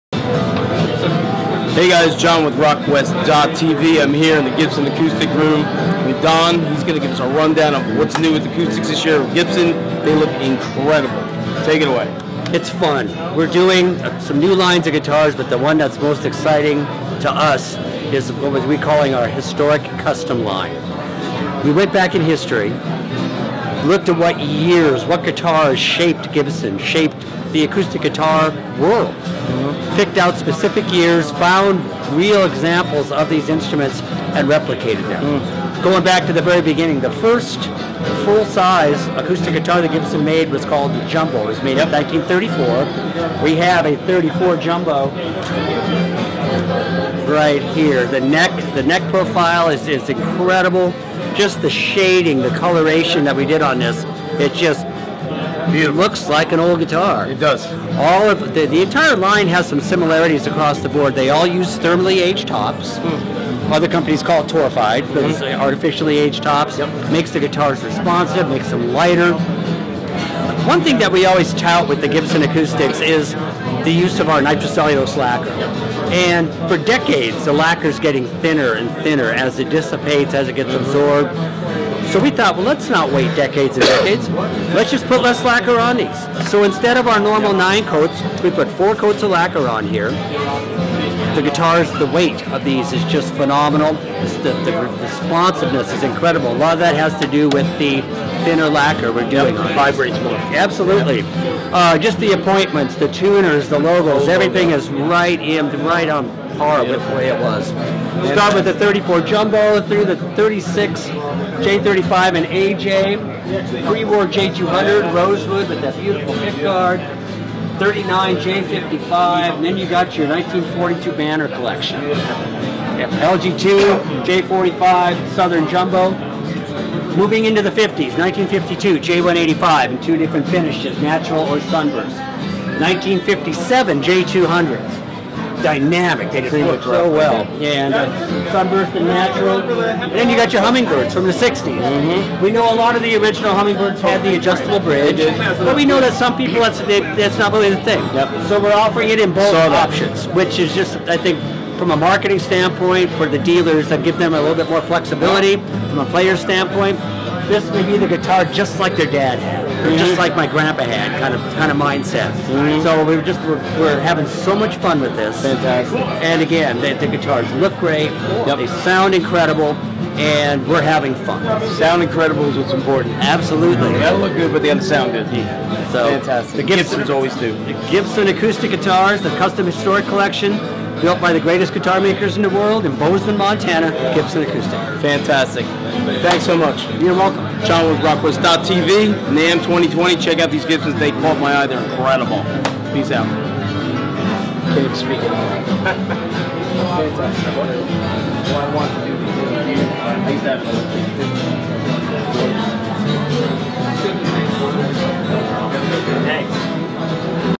Gibson acoustic guitars Historic Custom Line series showcased at NAMM 2020
Gibson custom shop released a new series of acoustic guitars that are designed like older classics that shaped Gibson guitars, such at the '34 Jumbo, multiple years of J-series and the Hummingbird. Light weight woods, thermally aged tops, and fewer coats of lacquer recreate the sound and feel of historic classics.